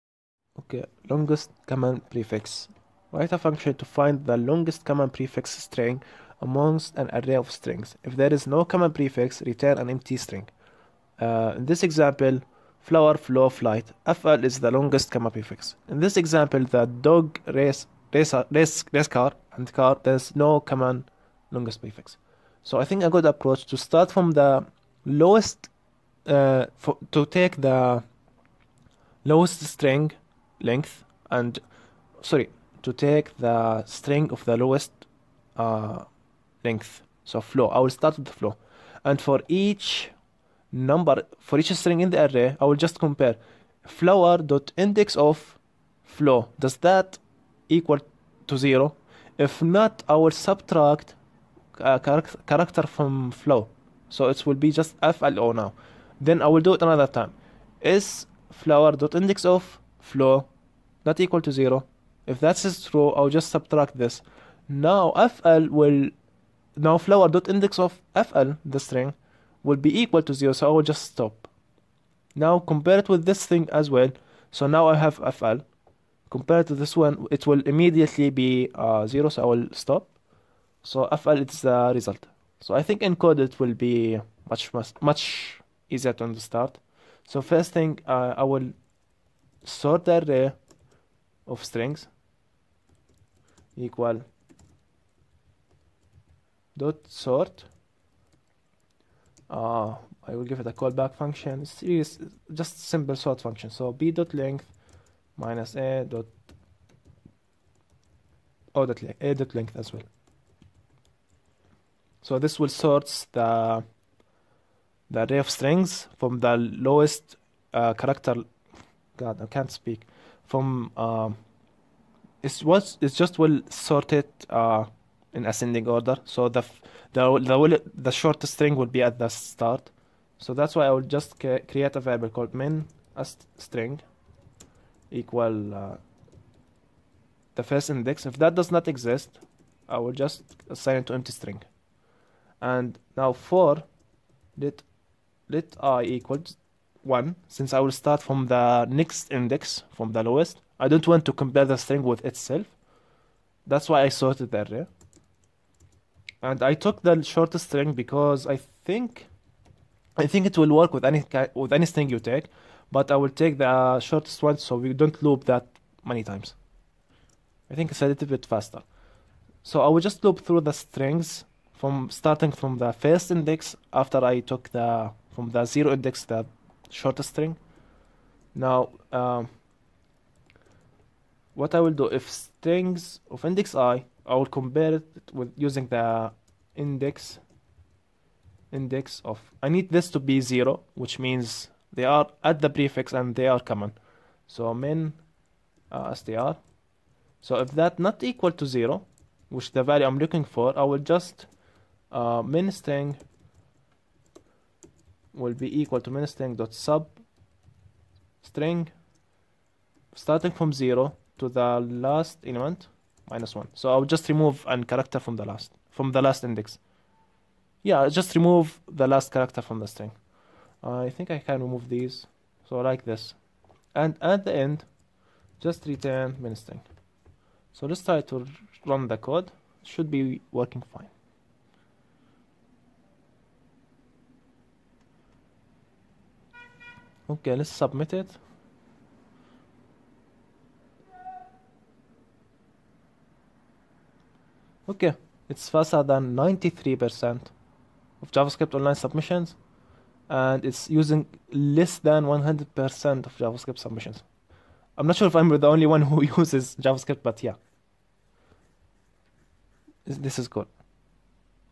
Solving "Longest Common Prefix" from LeetCode ( Easy ) - Problem solving in JS
I'm solving this problem in JS

* It's just a way for me to keep solving questions... *